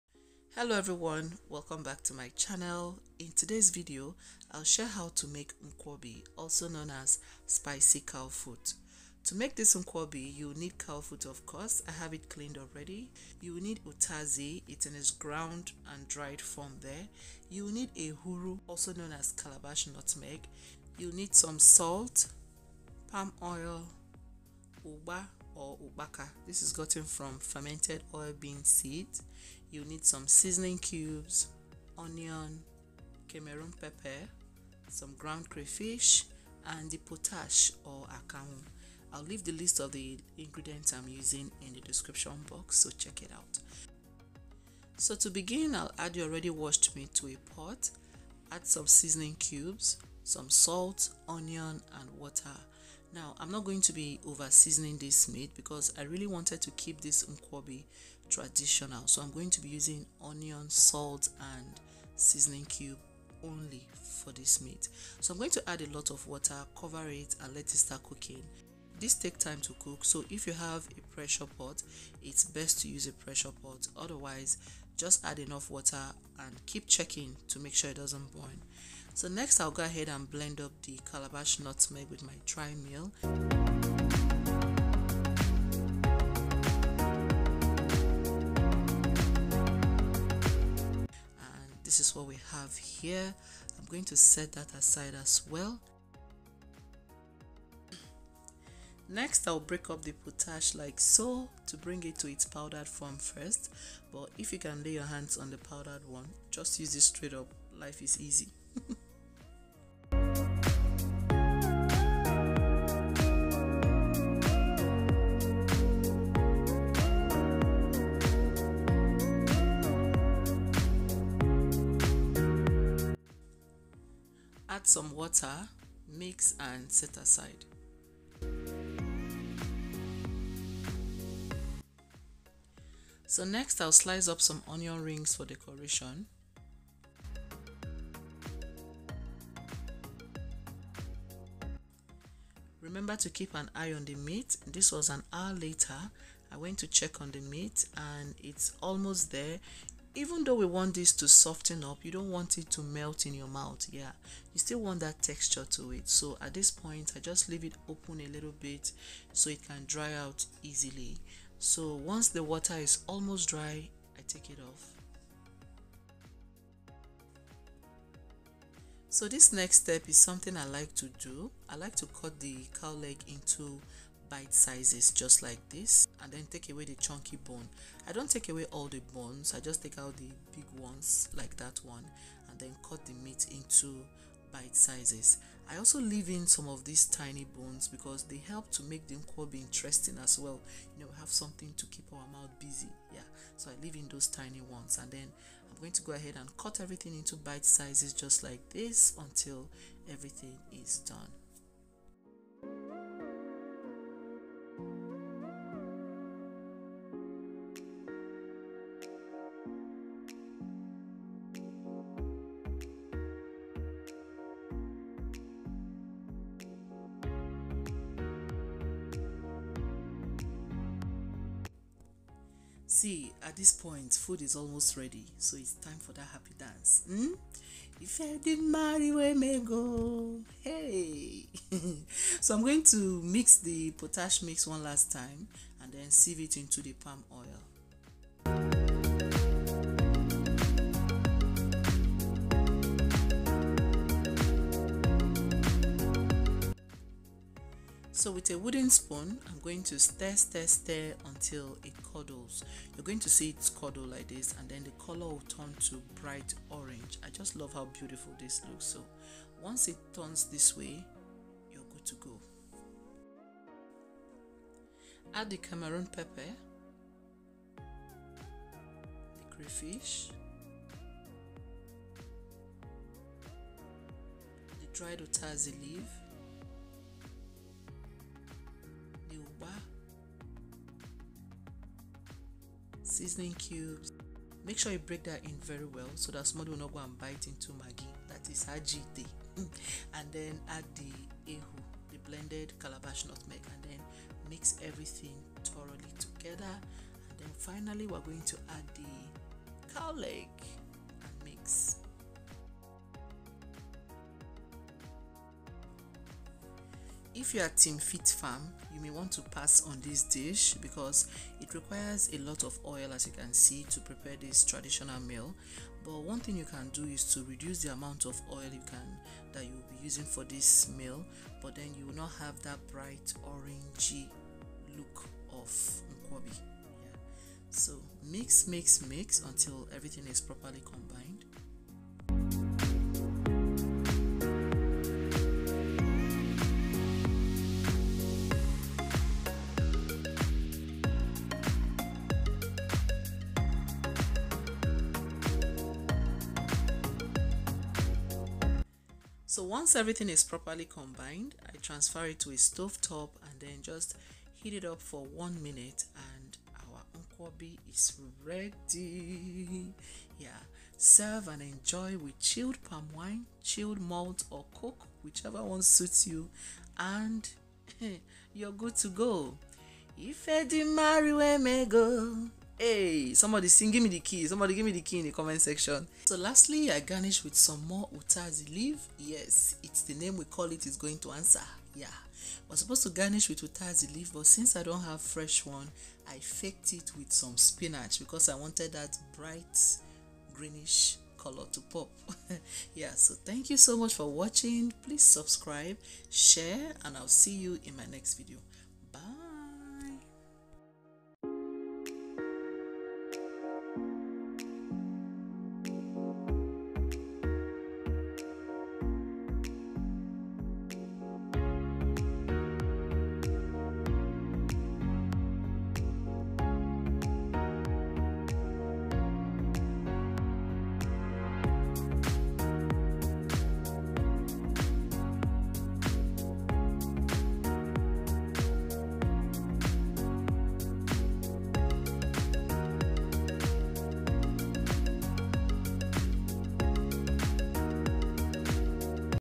How To Make Nkwobi | Full Option Nkwobi Recipe | Spicy Cow Foot
How to make Nkwobi ( spicy cow foot recipe $

Smoked cow foot 24 pieces
Ehuru ( calabash nutmeg) 12
1 onions
Cameroon pepper ( to taste)
Seasoning cube 2
Salt to taste
Crafish 6 full tablespoon
Palm oil 11/2 cups
Ugba/ ukpaka 1 full cup

Serve and enjoy with a chilled drink
You can also enjoy this with boiled yams.